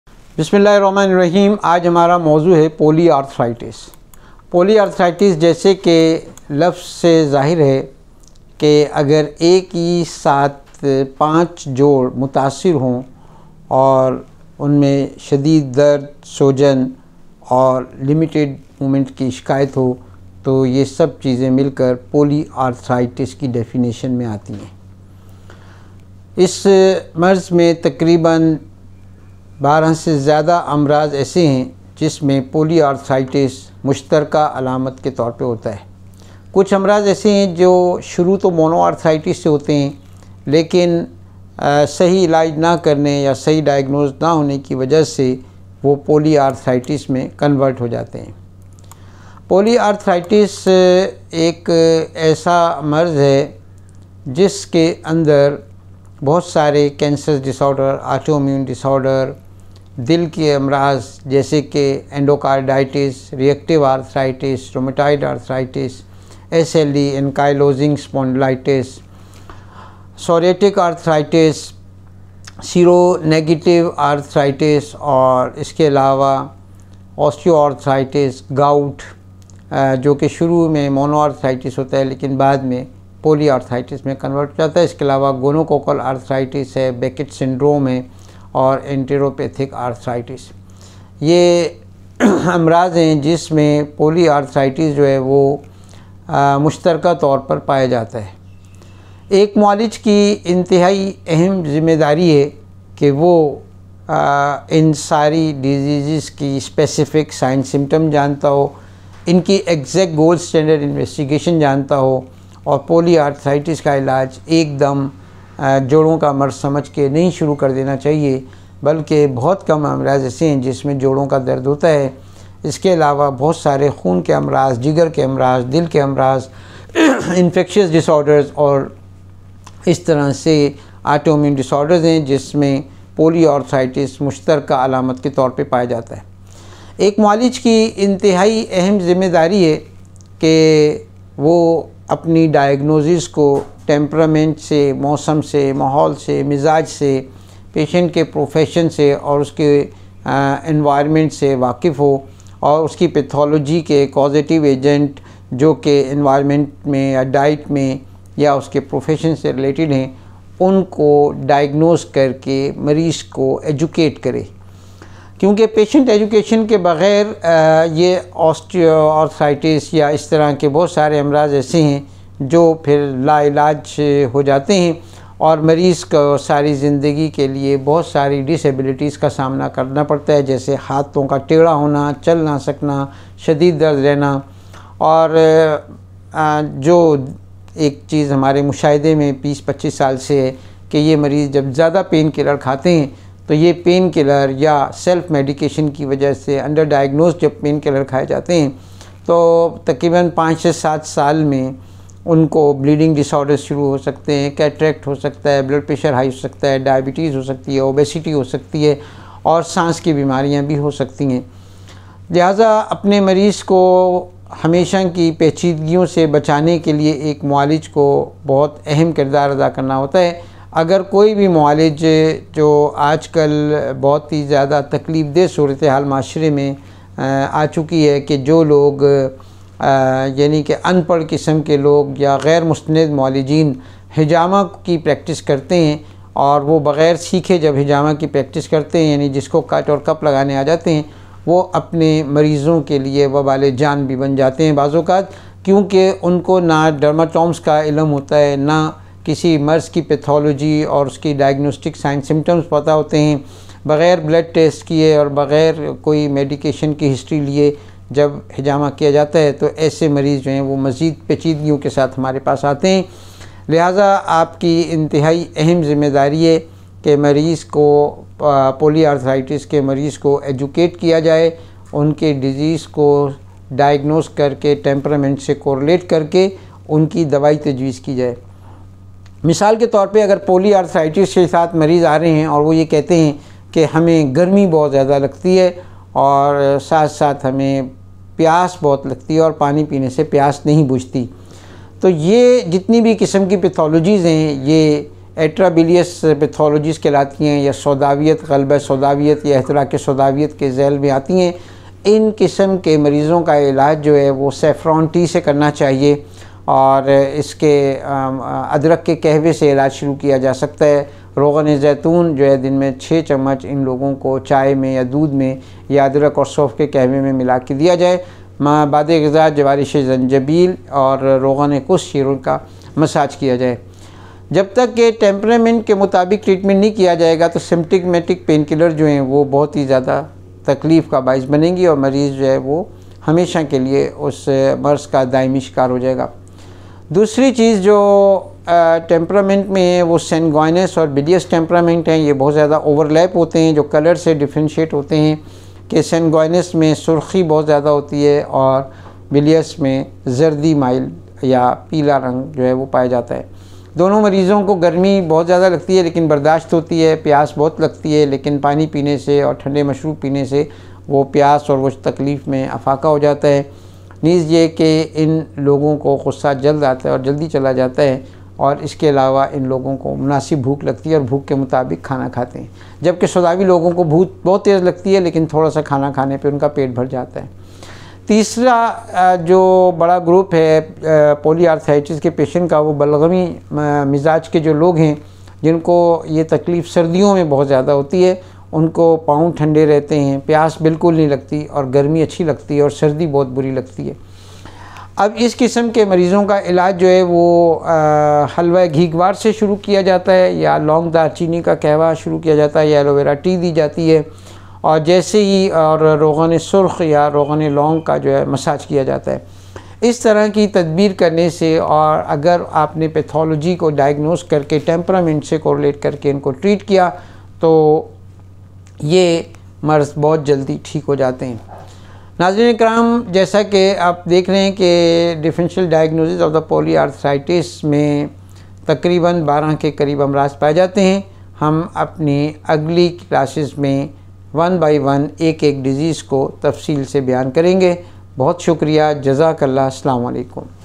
POLYARTHIRITIS | LECTURE | PART 1 PROF DR ASIF IQBAL
Polyarteritis nodosa is a serious inflammatory blood vessel disease. The small and medium-sized arteries become swollen and damaged. Polyarteritis nodosa, seen here on a lower extremity, produces painful, erythematous nodules. Arteries in the deep dermis and upper fat show neutrophilic infiltration.